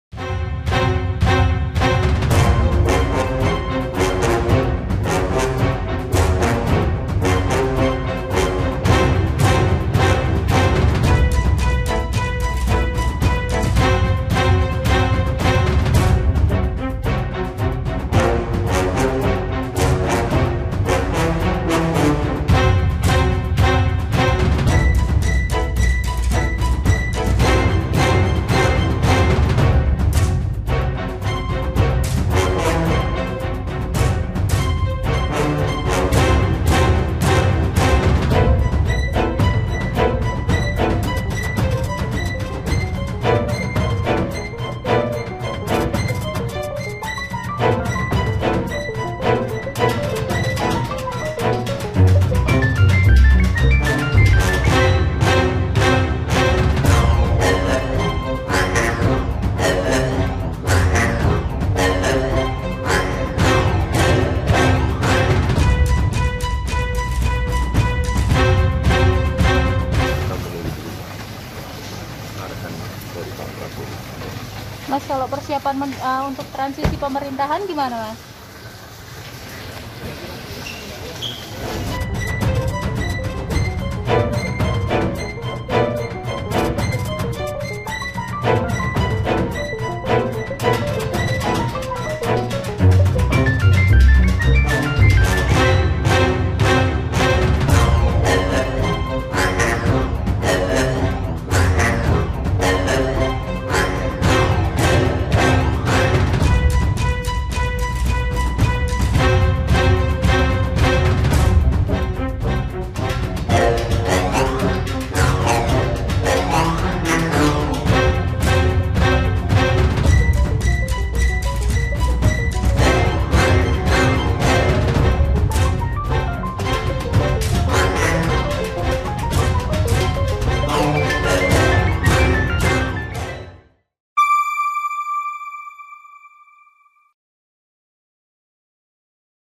Gibran Tanggapi Rencana PDIP Gugat KPU ke PTUN, Tunggu Arahan Pak Prabowo
Wapres terpilih Gibran Rakabuming Raka masih berkantor di Balai Kota Solo usai penetapan Capres dan Cawapres. Gibran akan fokus untuk menyelesaikan tugas-tugasnya sebagai Wali Kota Solo. Terkait PDIP yang akan menggugat KPU terkait proses Pilpres 2024 ke PTUN, Gibran menegaskan menunggu arahan Prabowo.

Kontributor: Septyantoro